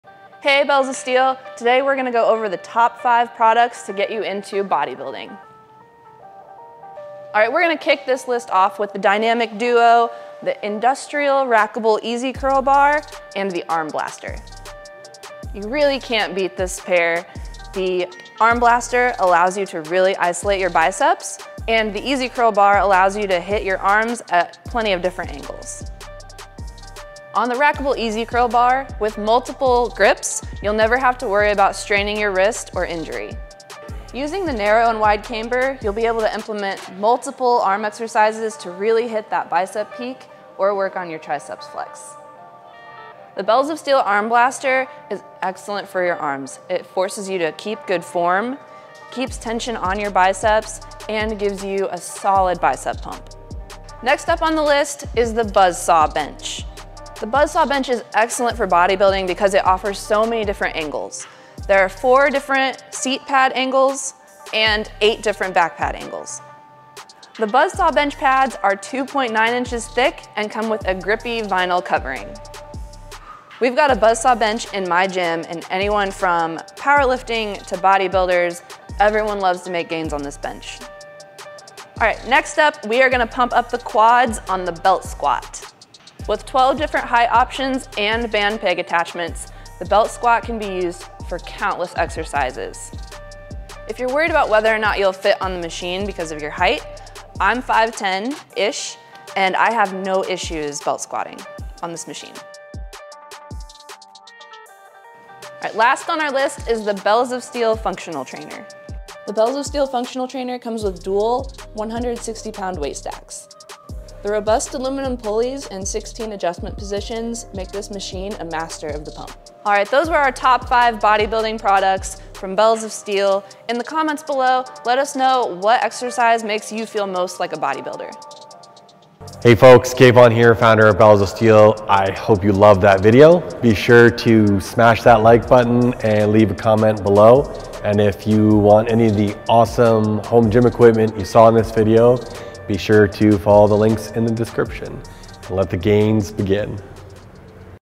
Top 5 Essential BELLS OF STEEL products for Home Gym BODYBUILDING
This video highlights the best Bells of Steel has to offer the home gym bodybuilding enthusiasts. These are the top 5 essential products.

0:00 POSE DOWN
0:10 Dynamic Duo
0:27 Rack-able Industrial E-Z Curl Bar
0:43 B.O.S Arm Blaster
0:55 The BUZZ SAW Bench
1:22 Belt Squat
1:45 Bells of Steel Functional trainer

Where can I get that belt squat?

Amazingly smooth functional trainer!

For more tips, deals and the best home gym equipment in the world give us a follow